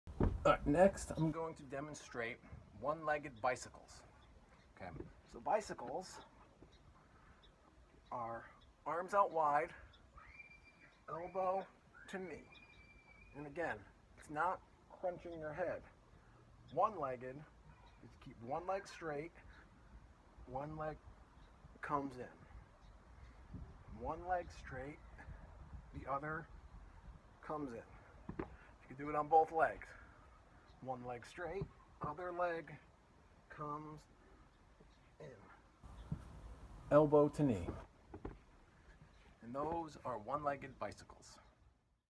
One Legged Bicycles
In this video Henk demonstrates One Legged Bicycles..